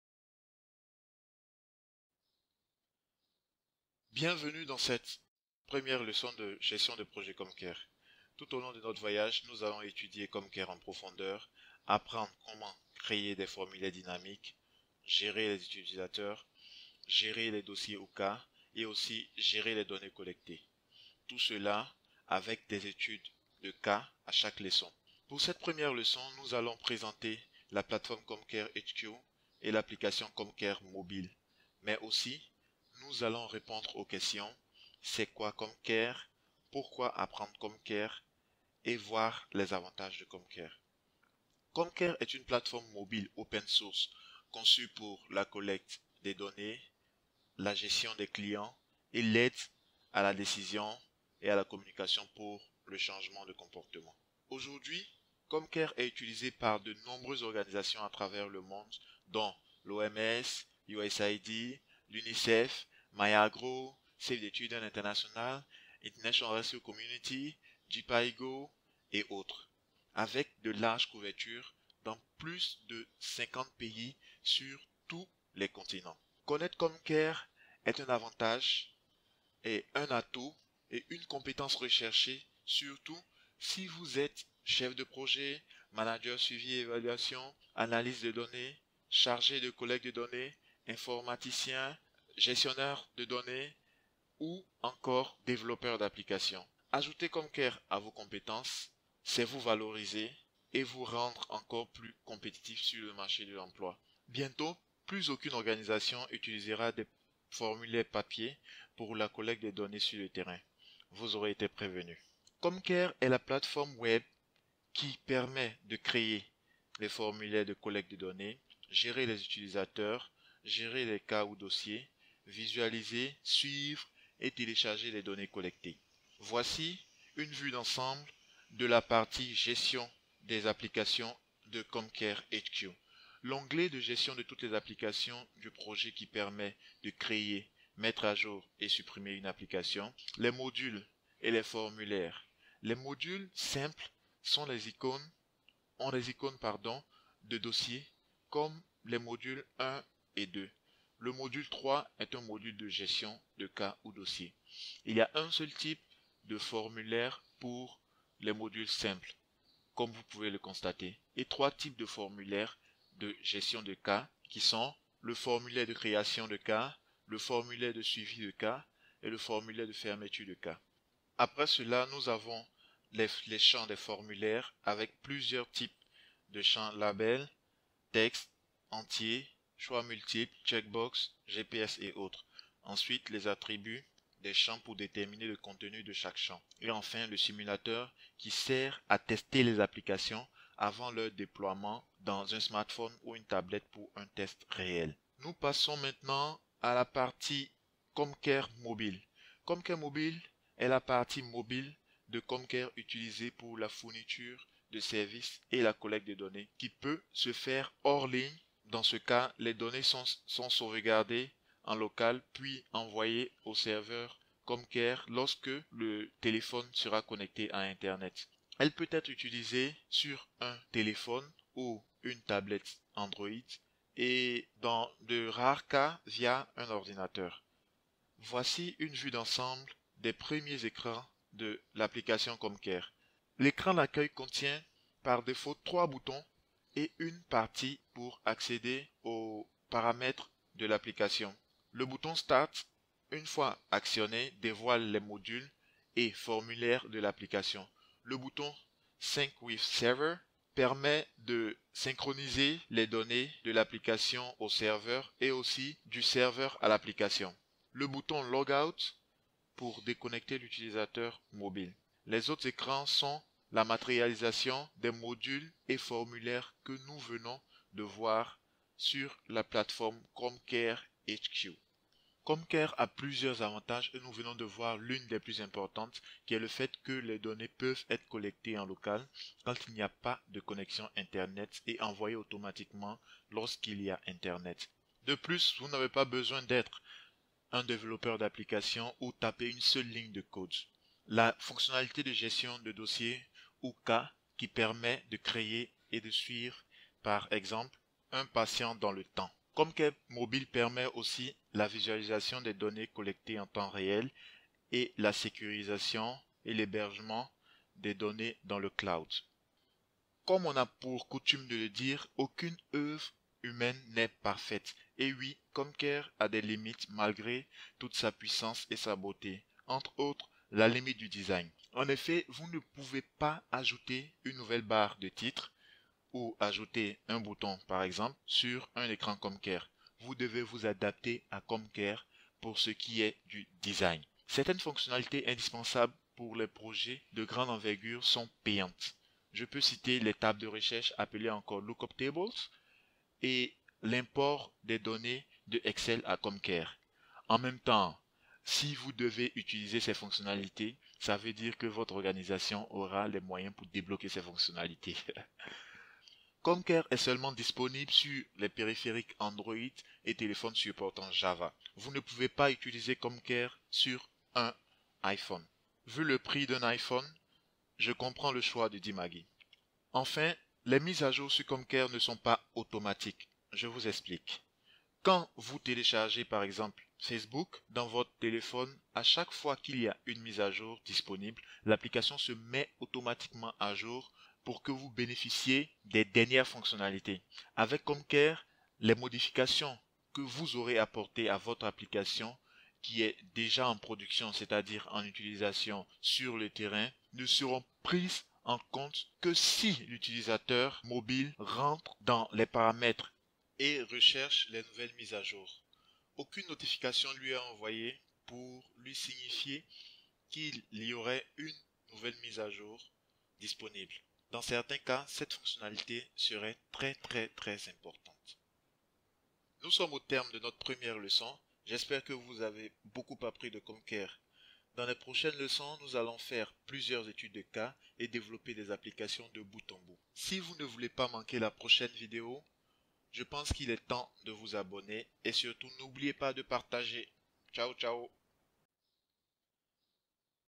Leçon 1 - Présentation de CommCare
Dans cette première leçon, nous verrons les éléments de base de CommCare:

C'est quoi CommCare ?
Pourquoi apprendre CommCare ?
Les avantages de CommCare
Les limites de CommCare